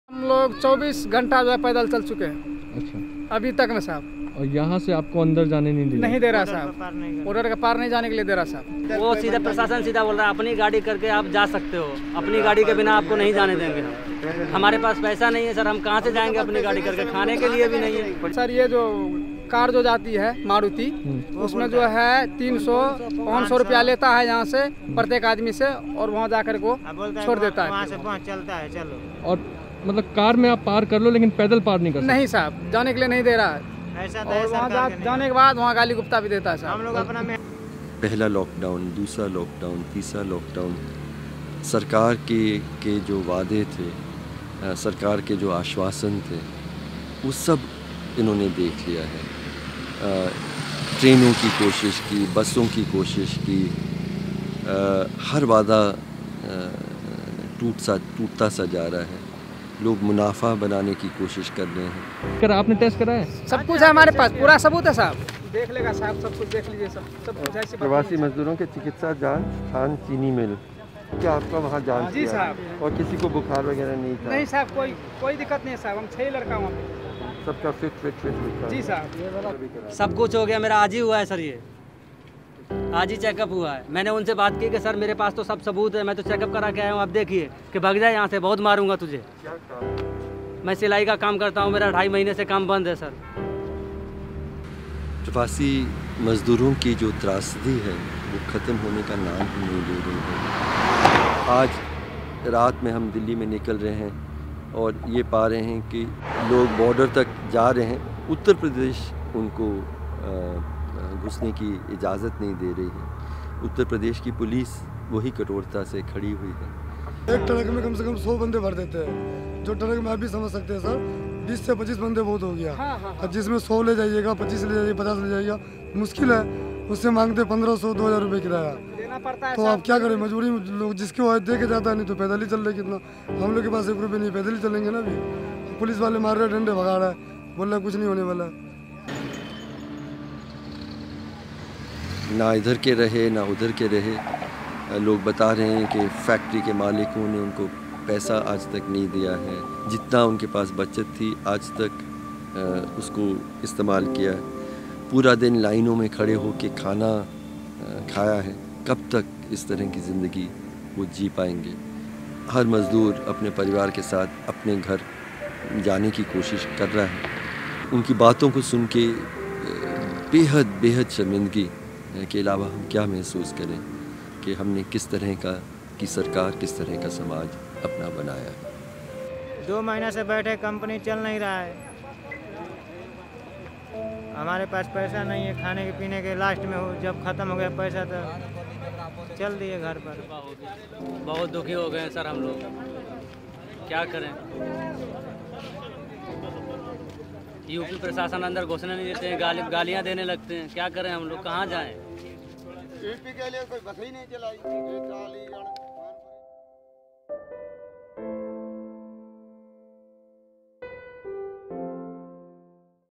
India's Workforce: Beaten, Betrayed And Stranded | Lockdown | Karwan e Mohabbat
"The companies have been shut for over 2 months now. We were just living off the meagre savings that we have. We have no money left now and just want to return to our villages." says a migrant worker from Bihar who has been stopped at the Delhi - Uttar Pradesh border.

Last night on the streets of the capital city of India, the people of India found themselves beaten, betrayed and stranded. "I have got a medical certificate to prove I am fit...yet the police is abusing me and refusing to let me go home," shares a young man who works as a tailor, but lockdown has stripped him of all his rights.